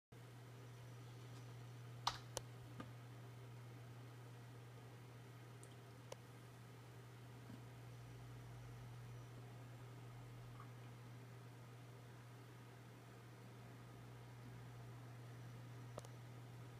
Pivot gun duel
A little animation I made when I was bored